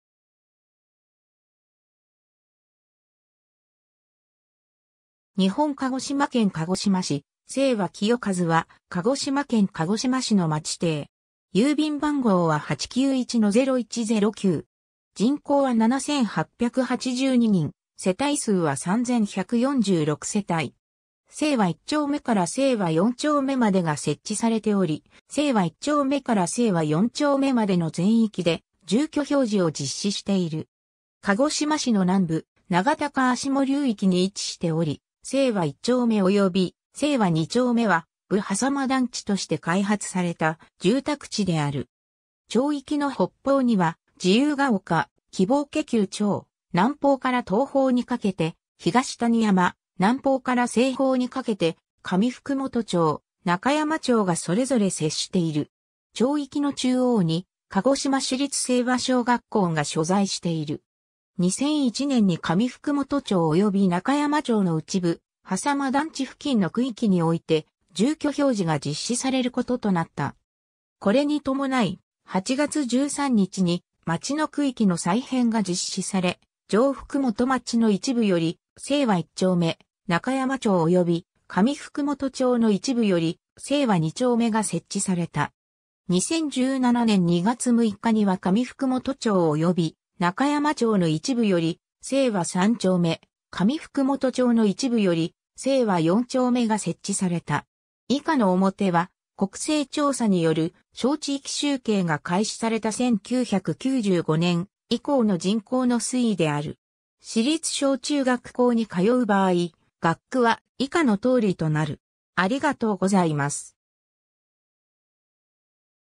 清和 (鹿児島市)
日本 」 鹿児島県 」 鹿児島市 」 清和 清和（せいわ）は、鹿児島県鹿児島市の町丁。
清和一丁目から清和四丁目までが設置されており、清和一丁目から清和四丁目までの全域で住居表示を実施している。
 鹿児島市の南部、永田川下流域に位置しており、清和一丁目及び清和二丁目は武迫団地として開発された住宅地である。
 町域の北方には自由ケ丘、希望ケ丘町、南方から東方にかけて東谷山、南方から西方にかけて上福元町、中山町がそれぞれ接している。
町域の中央に鹿児島市立清和小学校が所在している。
 2001年（平成13年）に上福元町及び中山町のうち武迫団地付近の区域において住居表示が実施されることとなった。
これに伴い、8月13日に町の区域の再編が実施され上福元町の一部より「清和一丁目」、中山町及び上福元町の一部より「清和二丁目」が設置された。
 2017年（平成29年）2月6日には上福元町及び中山町の一部より「清和三丁目」、上福元町の一部より「清和四丁目」が設置された。